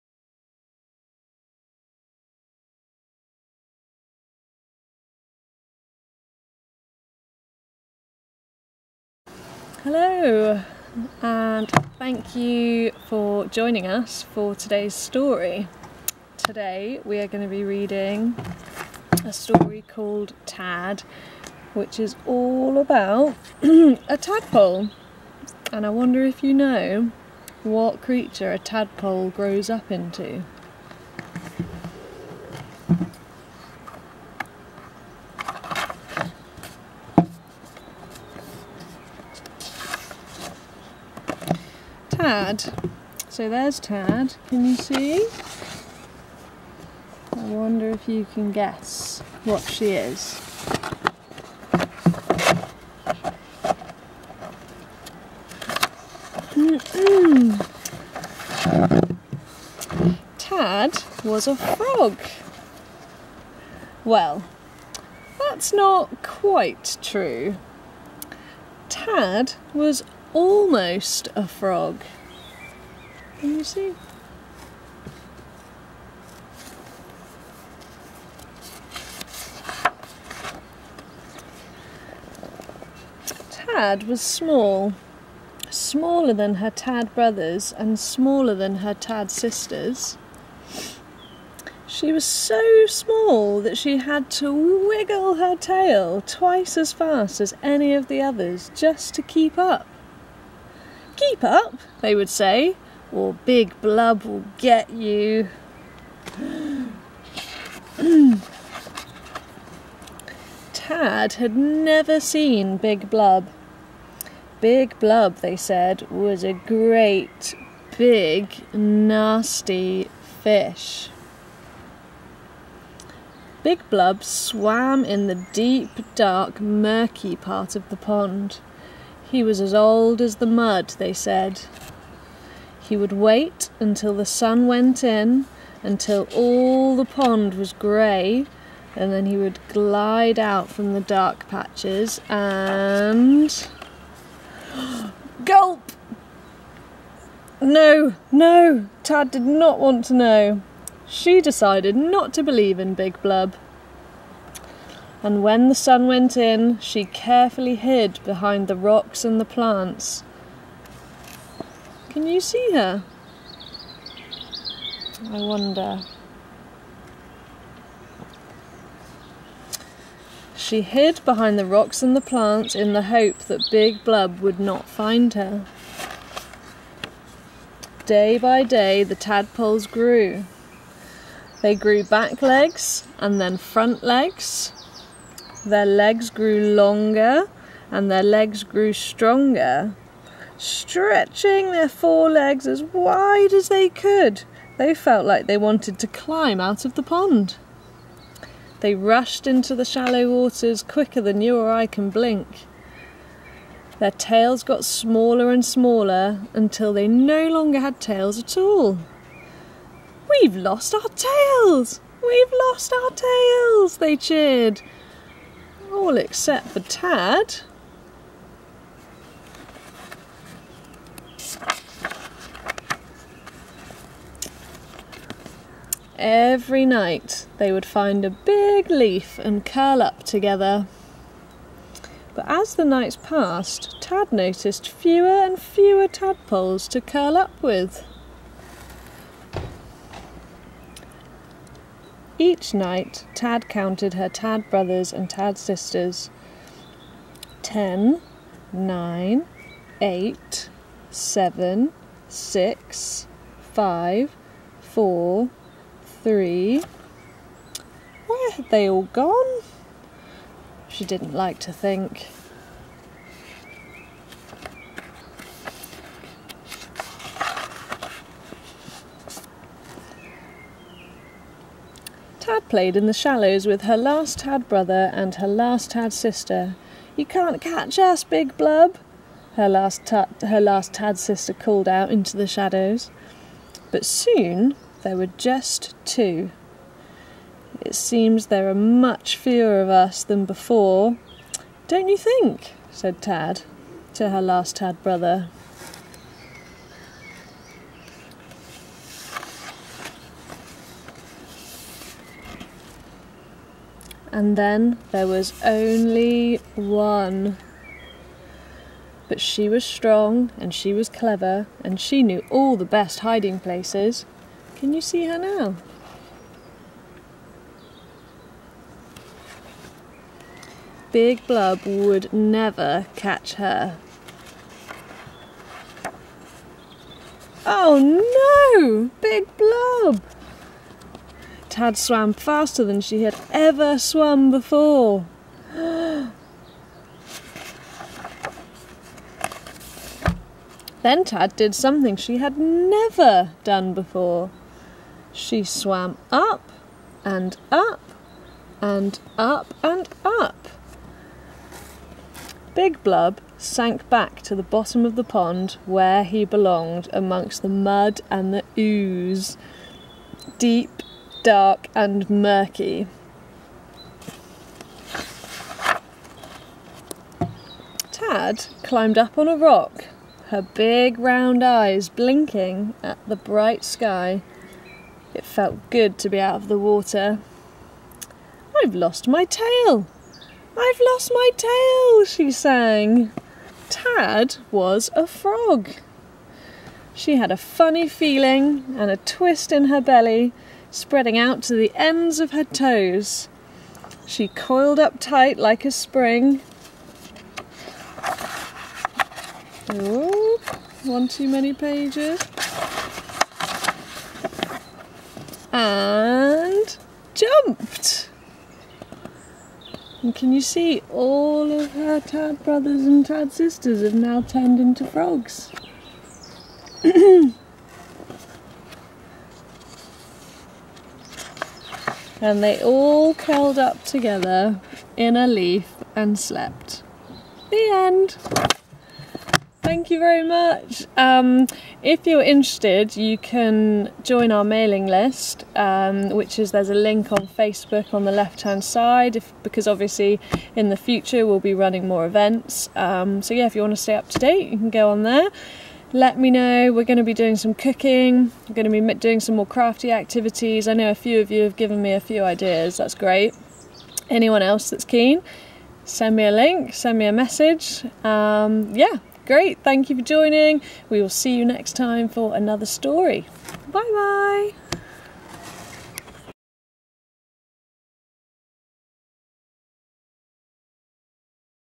Heather reading Tad by Benji Davies
A lovely book about a little frog finding her legs! One of my favourite books that I have for Forest School. I hope you enjoy it!

Tad by Benji Davies